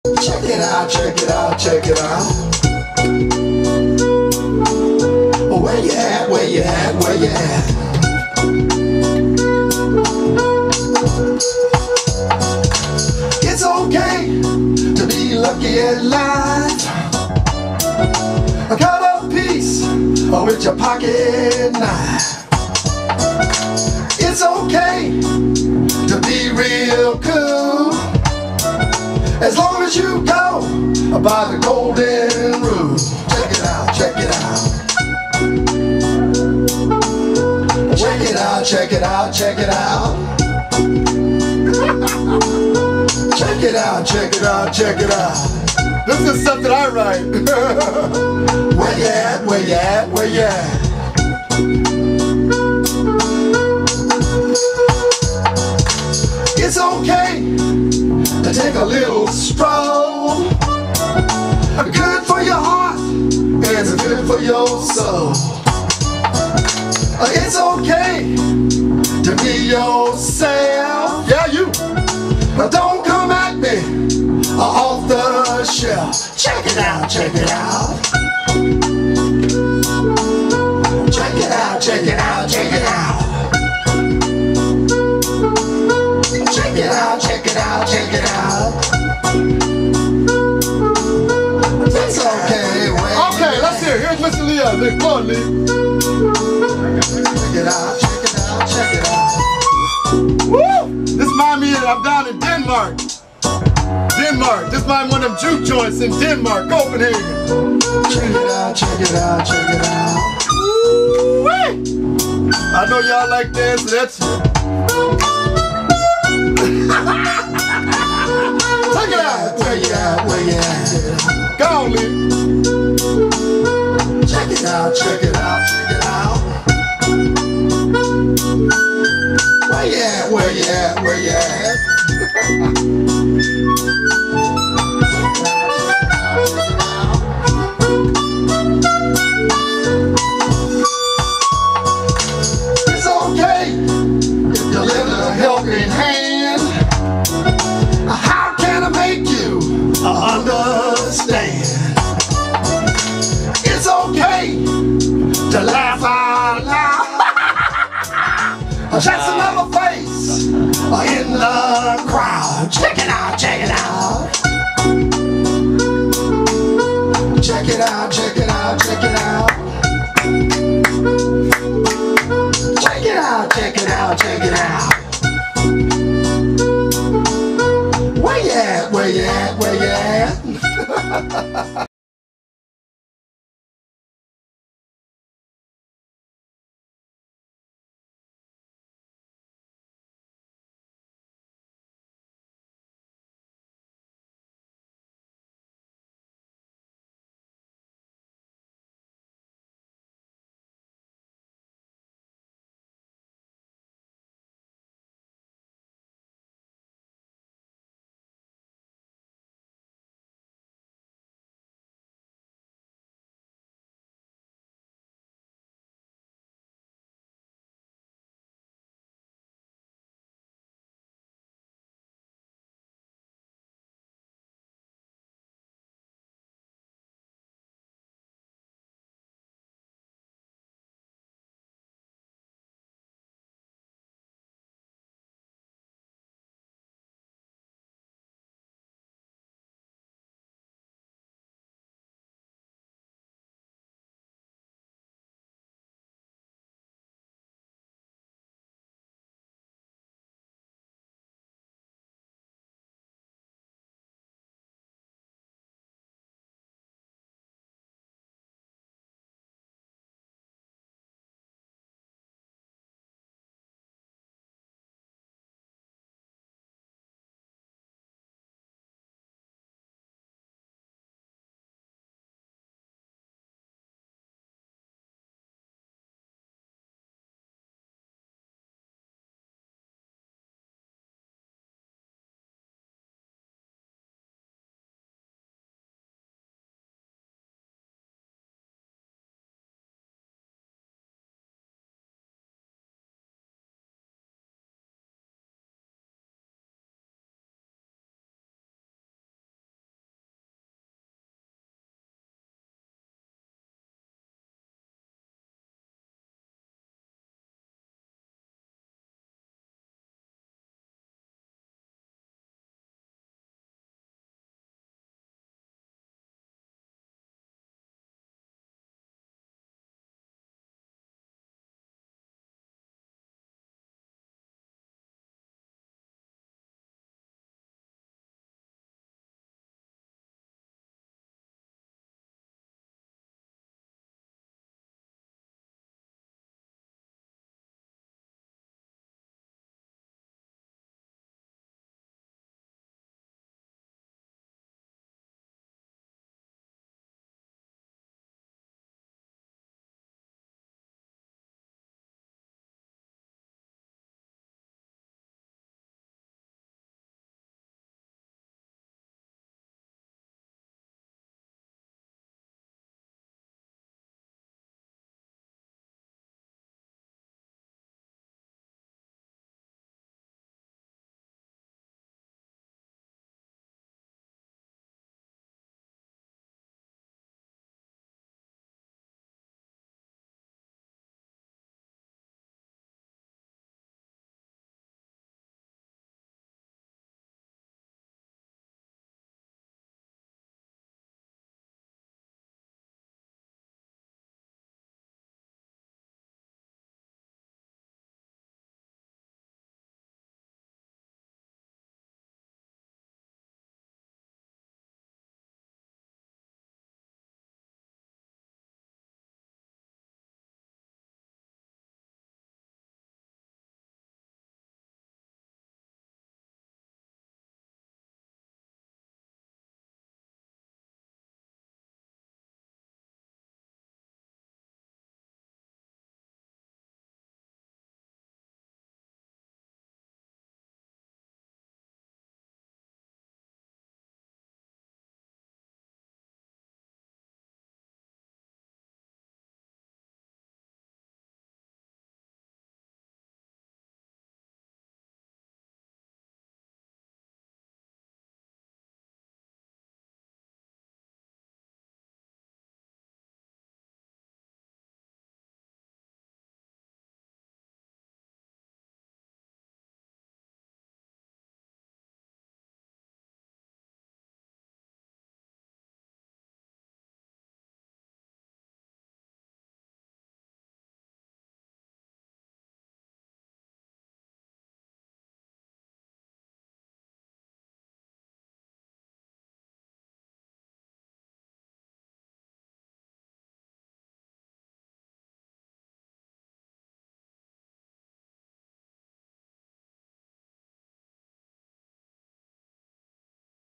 Harold Brown Check it Out
Harold Brown drummer-songwriter grooves to some of his new music